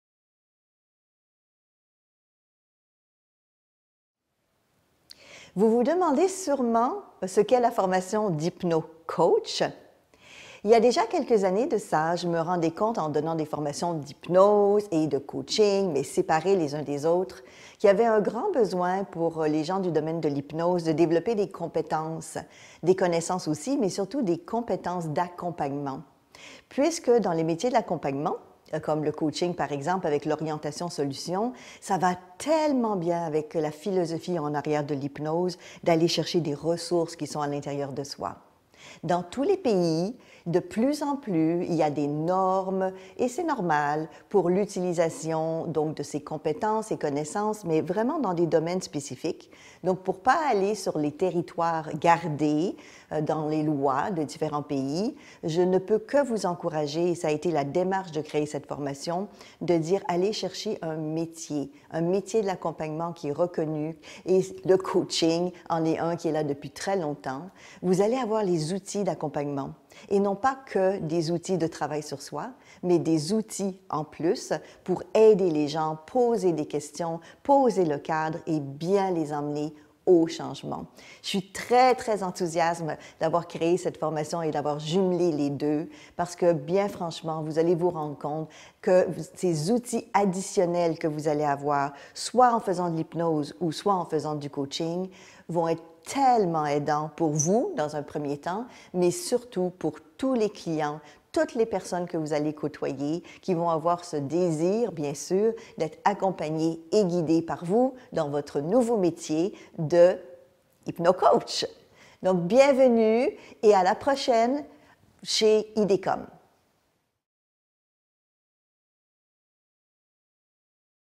Découvrez la formation d'Hypno Coach chez IDCom International
Découvrez la formation d'Hypno Coach chez IDCom International avec Isabelle David et son équipe. Plus d'information sur le programme en suivant ce lien